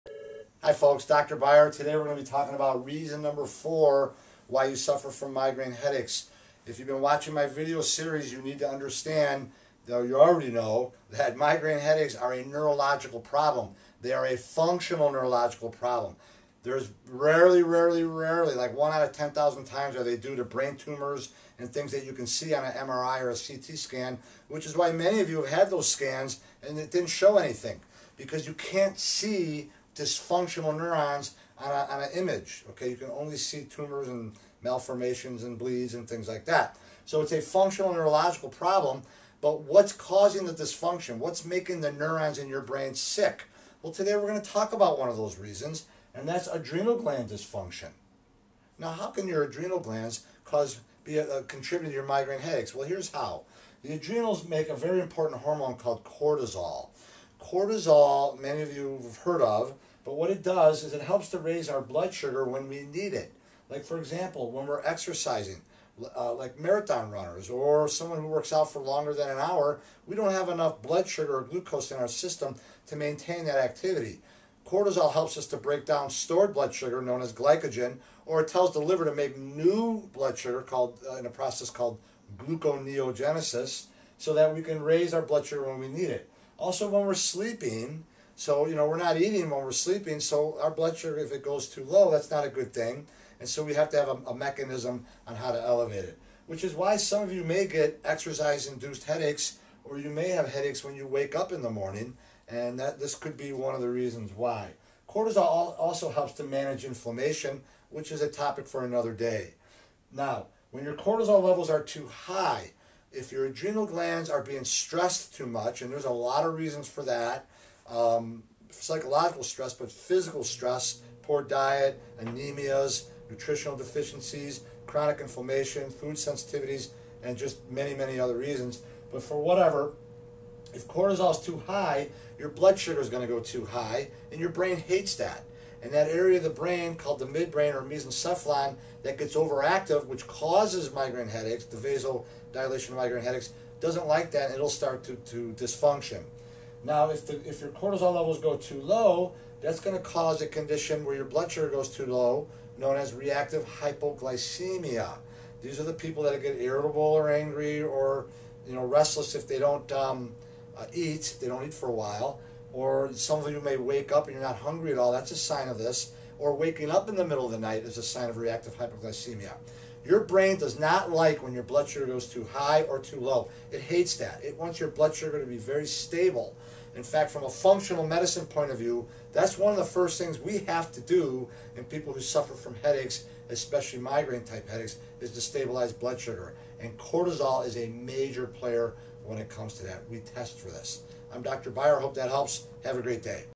Migraine Recovery #4: Adrenal Gland Dysfunction
What role does cortisol play in your migraines? Watch this short video to learn about the connection.